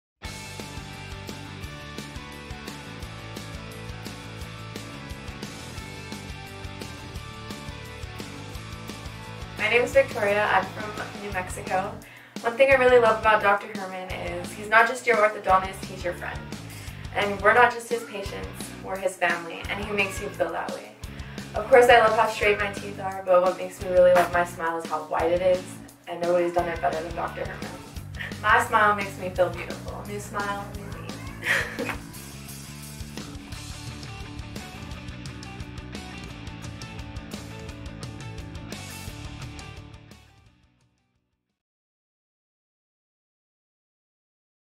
Victoria
“Dr. Herman is not only my orthodontist but was a friend to me and my family.” – Victoria

“Stunning is the best term to describe Victoria's transformation. Victoria is from Farmington, New Mexico. The key to enhancing Victoria's beauty was to widen her smile, create a beautiful smile arc and to bring her lower jaw forward. Other orthodontists would have just straightened her teeth and left her smile narrow. When choosing an orthodontist, compare results and the choice will be easy. Please don't let gimmicks and discounts cause you to lose focus on what is most important, "the result".” – Dr. Herman